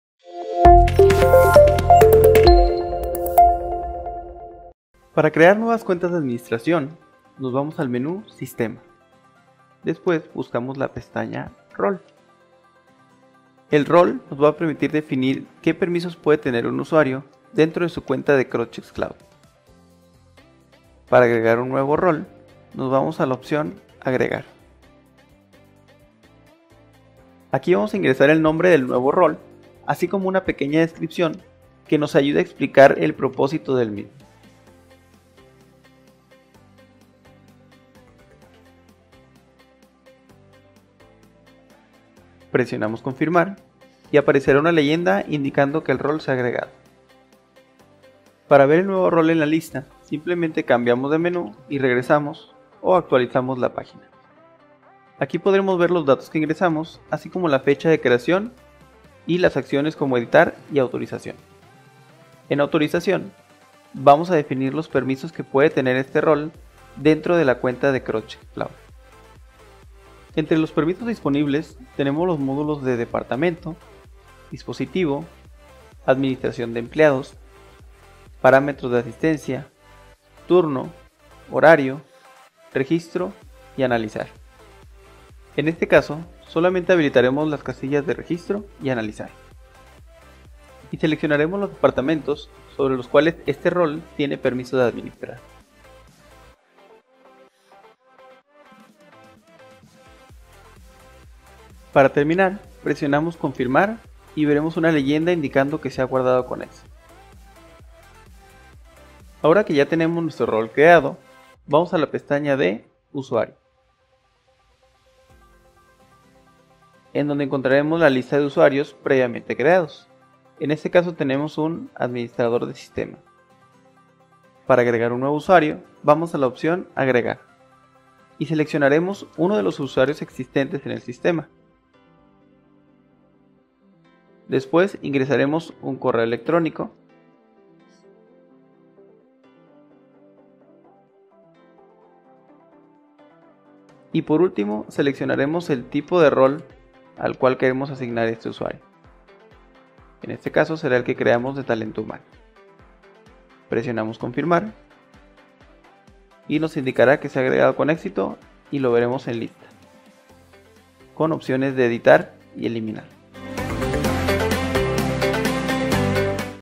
Crear cuenta administrativa crosschex cloud Anviz
Capítulo 11
En éste tutorial aprenderemos a crear una cuenta administrativa en el crosschex cloud de Anviz